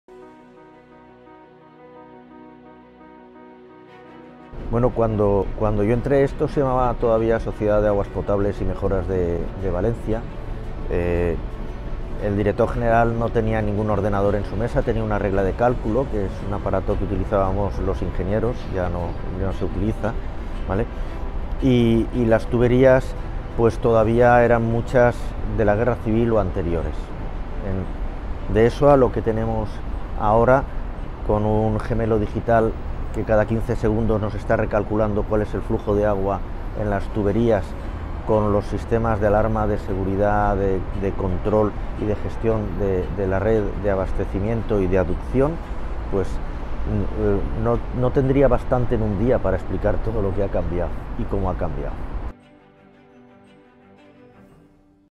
Diálogos del Agua con Jaime Castillo: Explica como ha cambiado Global Omnium
💧 Jaime Castillo Dtr. de I+D+i y RR.II de GlobalOmnium hace un recorrido por la historia de Global Omnium: de la regla de cálculo y las herrakientas de principios de siglo XX al Gemelo Digital que cada 15 segundos recalcula el flujo de la red 💧